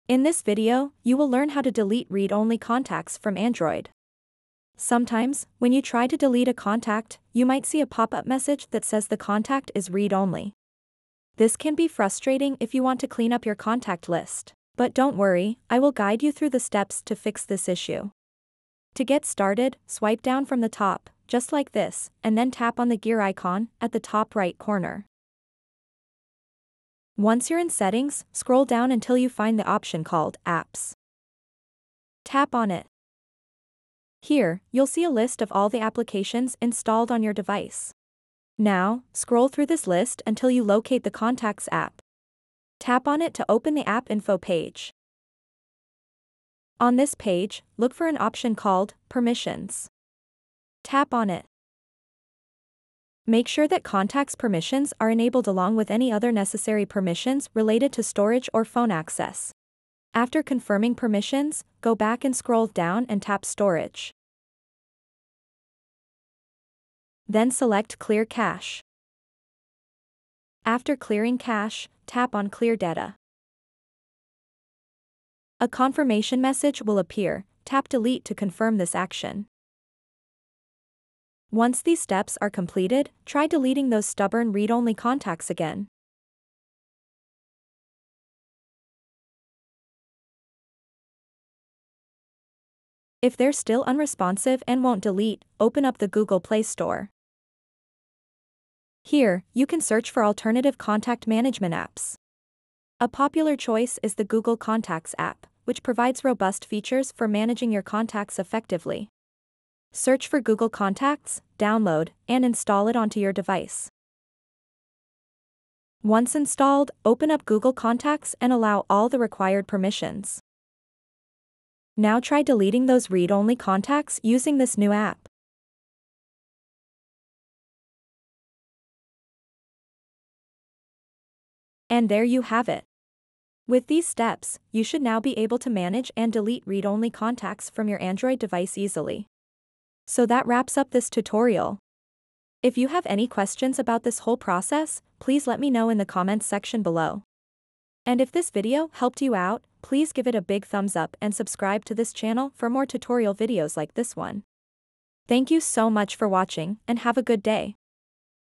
How To Delete Read Only Contacts From Android
In this video, you will learn How To Delete Read Only Contacts From Android. Sometimes, you may find that certain contacts cannot be deleted because they are set to read-only. This means you can view them, but you can’t remove them like regular contacts. We will guide you through the steps to get rid of these read-only contacts easily.